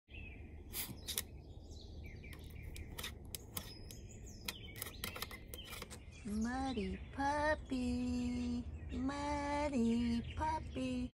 You DIRTY DOGS - If there is a MUD PUDDLE, A Westie will always find it
In SC we have clay-like red mud that stains everything.  But, Westies are gonna Westie, right?  3 Mo Old Skye and 8 Mo Old Echo don’t let a little mud stop their fun.